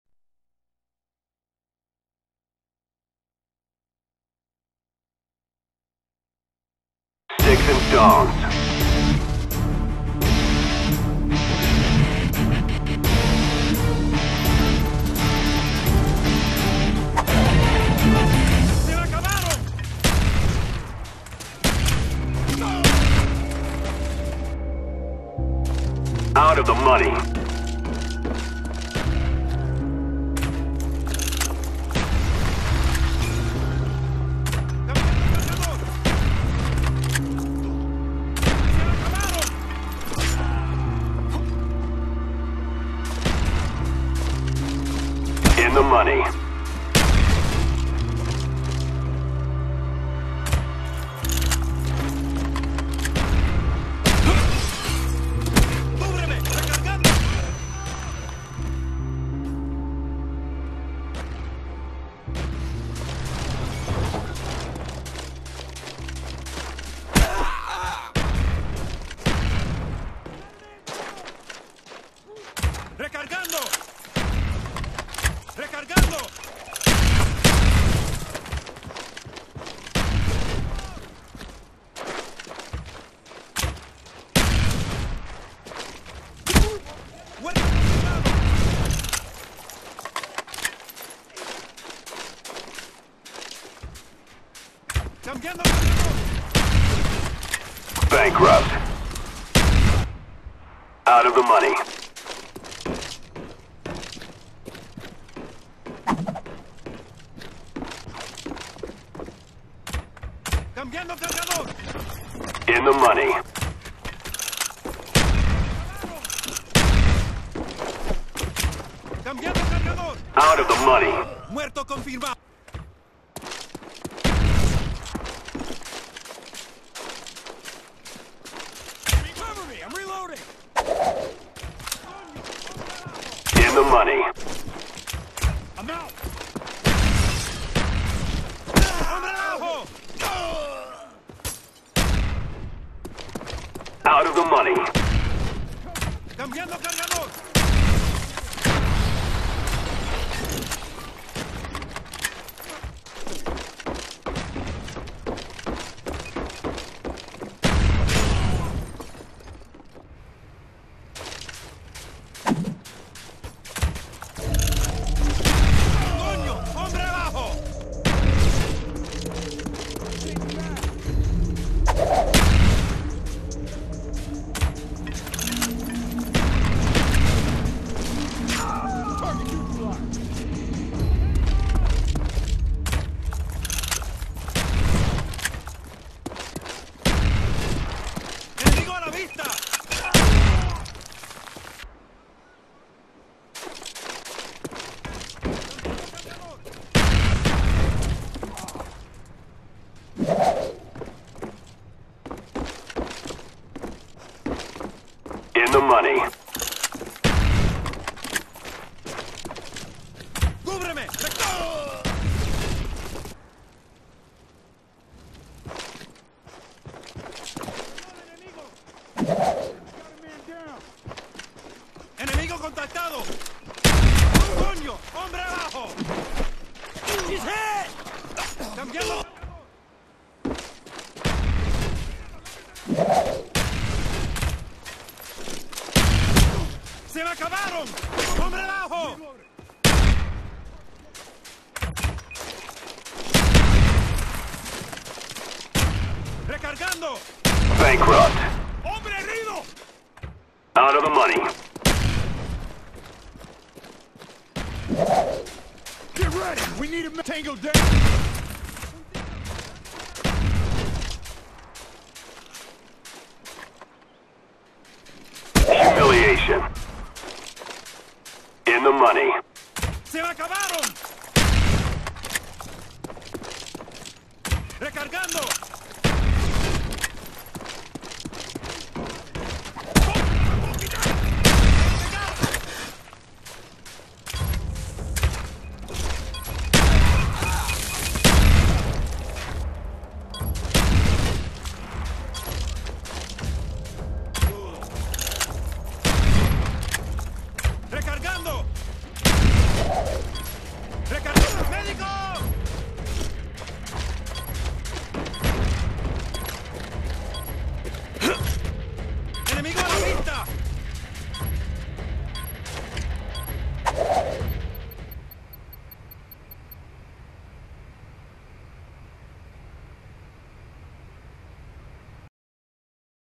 Boards.ie Wager Match
Just a test of the HD-PVR, I didn't even do that well in this match. I'm probably going to do a few commentaries after this.